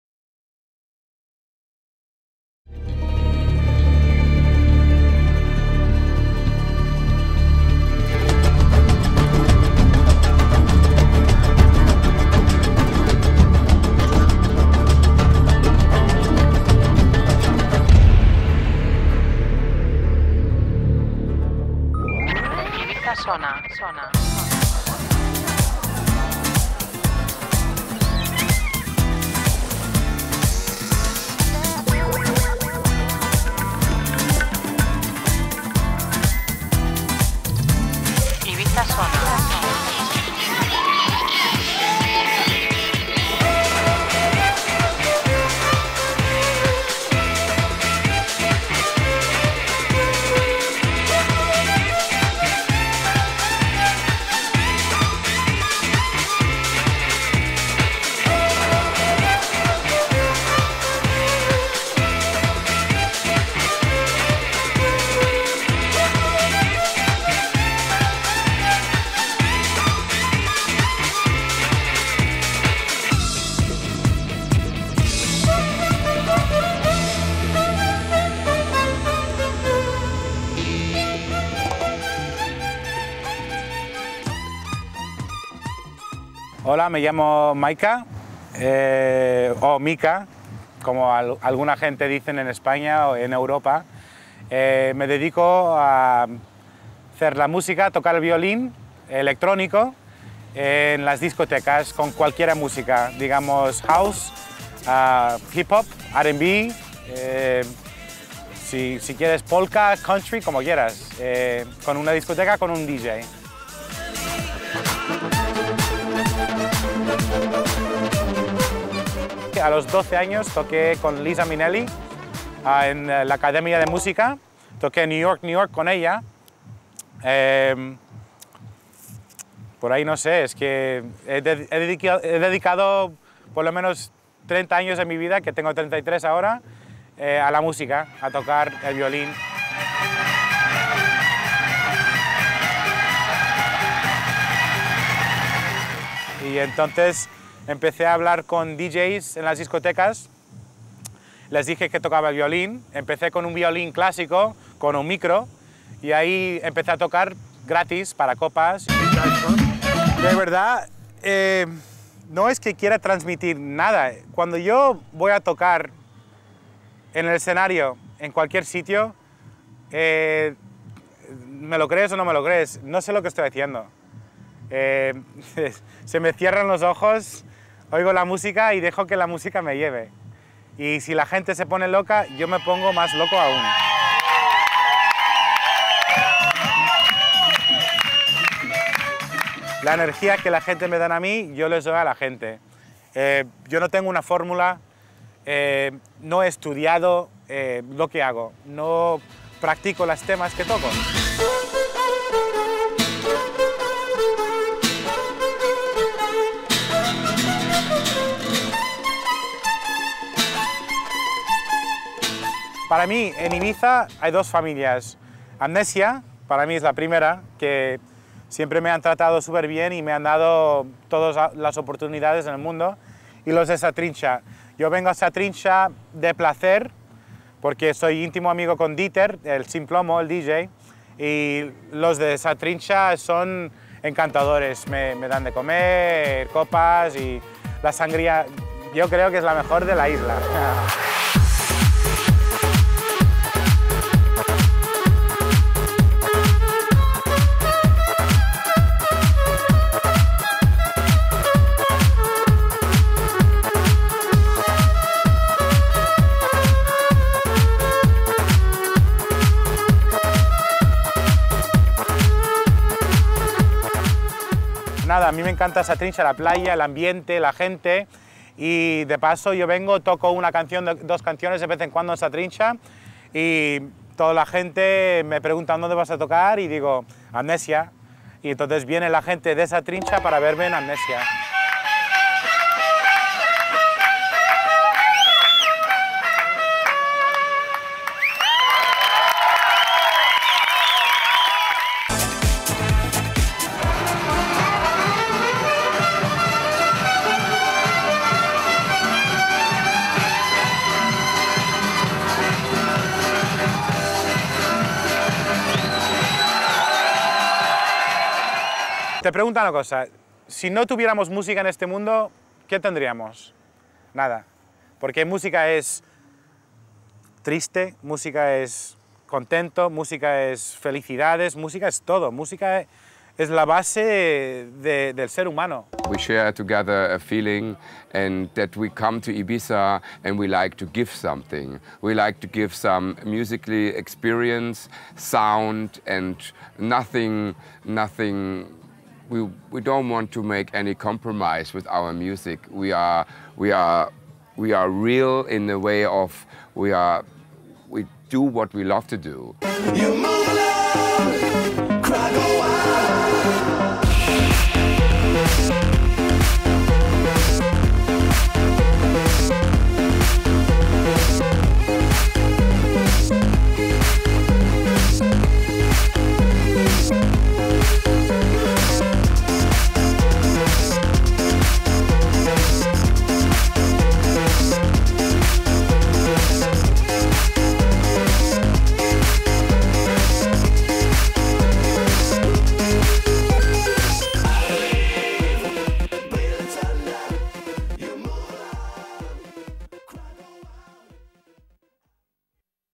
Micah the violinist - Amnesia Ibiza - IBZ Sona Interview
Micah - Amnesia Ibiza - IBZ Sona Interview